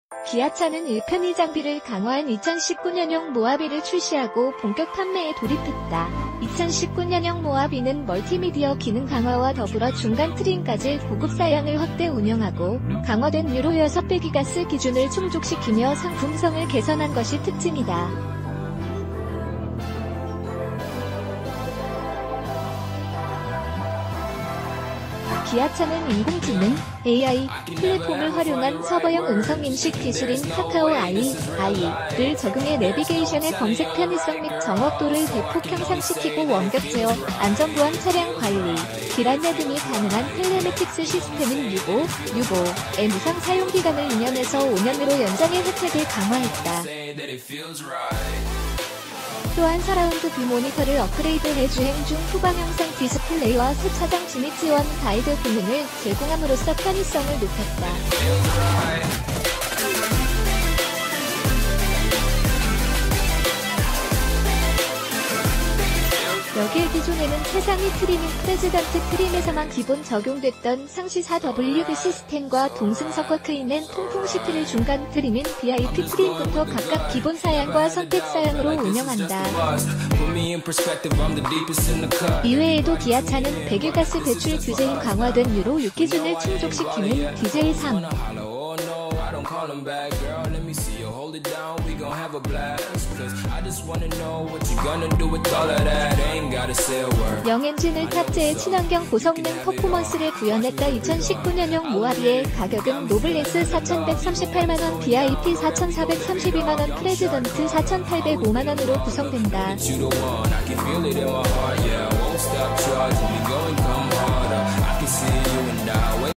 기아차, 편의장비 강화한 '2019 모하비' 출시 모토야|카24/7
기아차는 8일,편의장비를 강화한2019년형 모하비를 출시하고 본격 판매에 돌입했다.

2019년형 모하비는 멀티미디어 기능 강화와 더불어 중간트림까지 고급사양을 확대 운영하고. 강화된 유로6 배기가스 기준을 충족시키며 상품성을 개선한 것이 특징이다.기아차는 인공지능(AI)플랫폼을 활용한 서버형 음성인식 기술인 ‘카카오i(아이)’를 적용해 내비게이션의 검색 편의성 및 정확도를 대폭 향상시키고 원격제어,안전보안,차량관리,길안내 등이 가능한 텔레매틱스 시스템인 ‘UVO(유보)’의 무상 사용기간을2년에서5년으로 연장해 혜택을 강화했다.또한 서라운드 뷰 모니터를 업그레이드 해 ‘주행 중 후방영상 디스플레이’와 ‘세차장 진입지원 가이드’ 기능을 제공함으로써 편의성을 높였다. 여기에기존에는 최상위 트림인 ‘프레지던트’ 트림에서만 기본 적용됐던 ‘상시4WD’ 시스템과 ‘동승석 워크인&통풍시트’를 중간 트림인 ‘VIP’ 트림부터 각각 기본사양과 선택사양으로 운영한다.이외에도 기아차는 배기가스 배출 규제인 강화된 유로6기준을 충족시키는 디젤3.0엔진을 탑재해 친환경 고성능 퍼포먼스를 구현했다.2019년형 모하비의 가격은 ▲노블레스4,138만원 ▲VIP 4,432만원 ▲프레지던트4,805만원으로 구성된다.